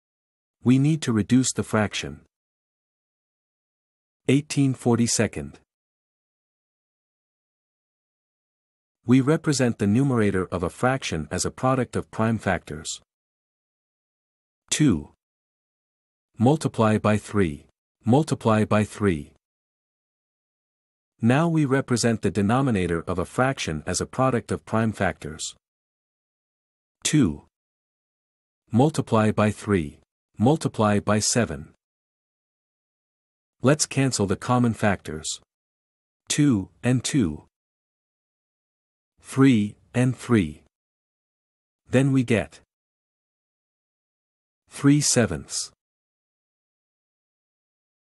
Reducing fractions. Example 03.
In this video, we will learn how to reduce fractions.